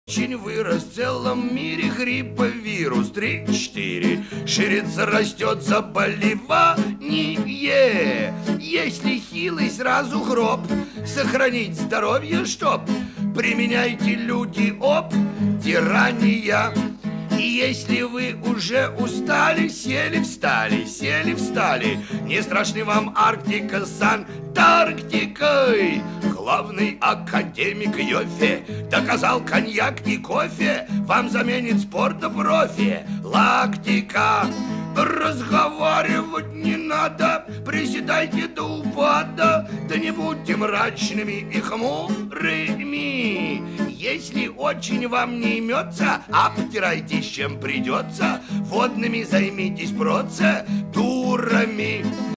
Эстрадный бард о ВЫРУСАХ !
поёт В. Высоцкий.